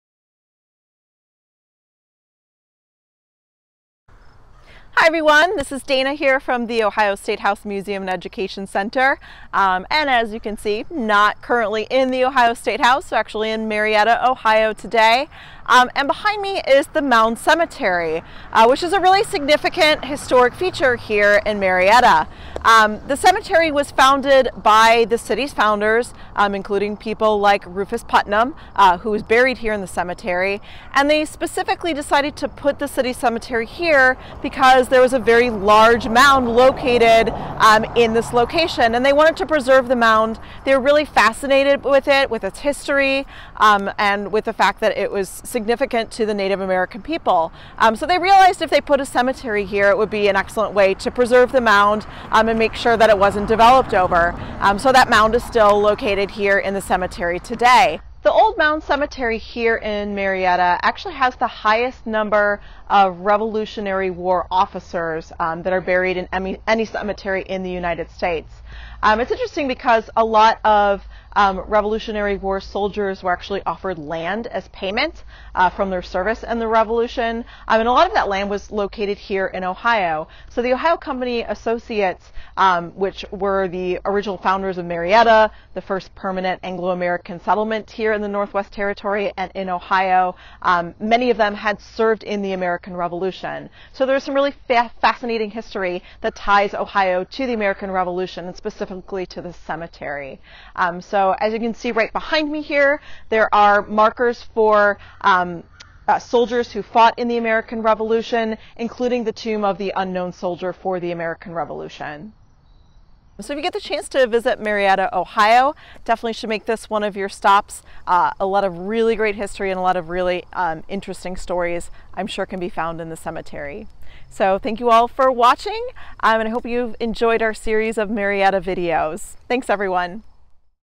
Marietta Part V: Mound Cemetery
In our final installment of the Marietta series, we take a visit to Mound Cemetery.